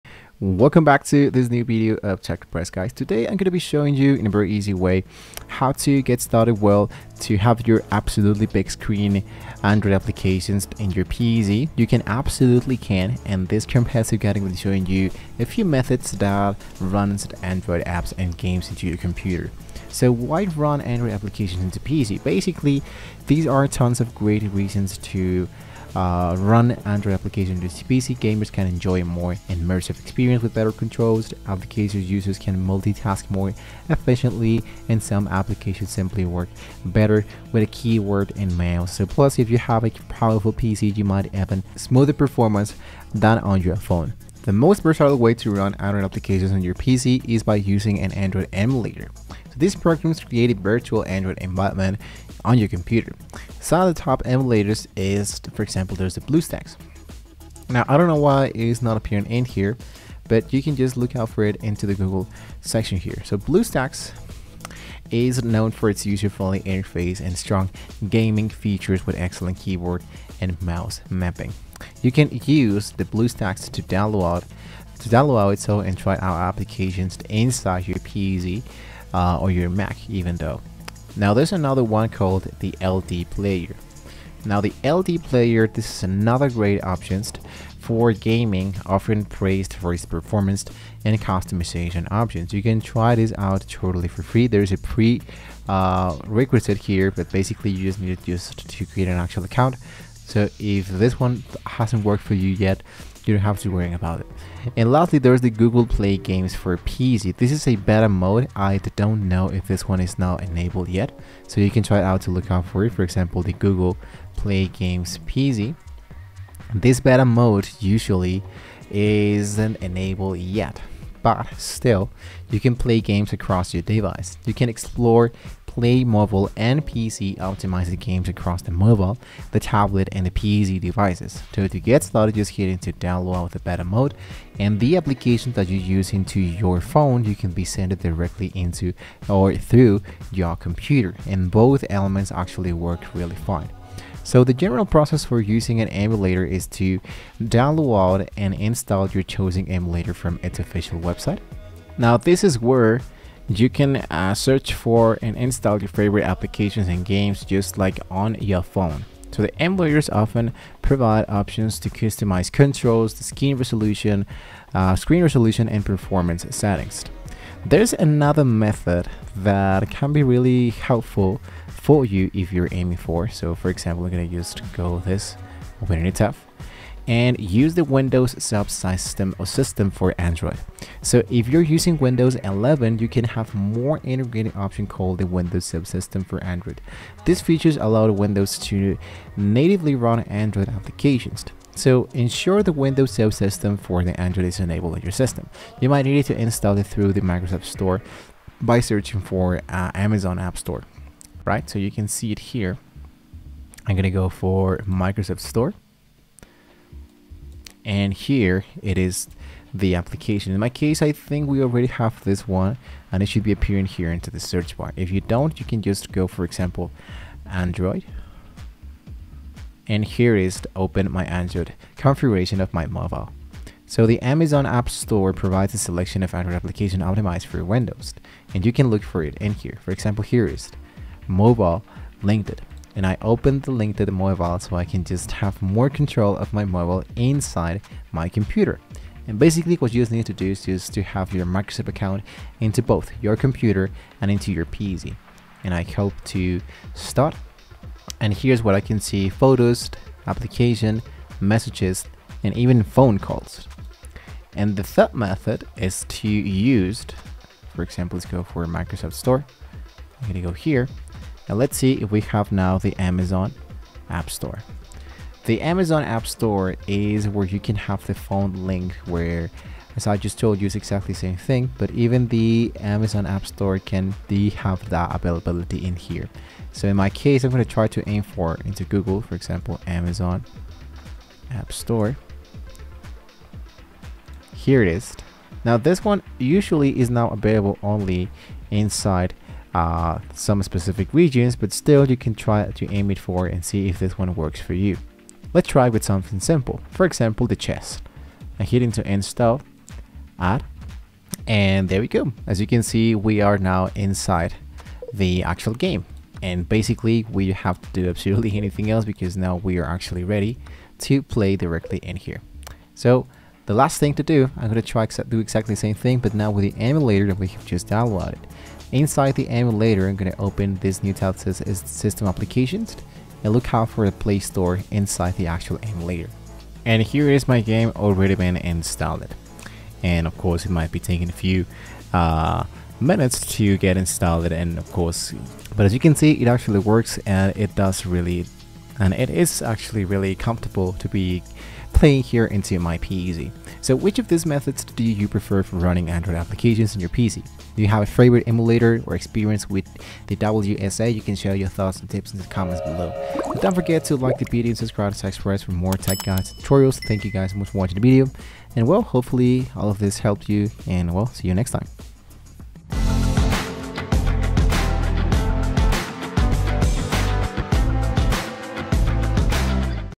How to Run Android Apps & Games on Your PC or Laptop (2026) Easy Guide
In this 2025 tutorial, I’ll show you exactly how to run Android apps and games on any computer—fast, easy, and for FREE!

🧩 In this video, you’ll learn:
✔️ The best emulators and tools available in 2026
✔️ How to install and set everything up step-by-step
✔️ How to optimize performance for gaming
✔️ Tips for compatibility, controls, and settings

🎮 Works great for:
- WhatsApp, Instagram, and other Android apps
- Mobile games like Free Fire, PUBG, Genshin Impact, and more
- Developers testing Android apps

✅ Compatible with Windows 10/11 and Mac systems
🔥 No lag – No complicated setup – 100% FREE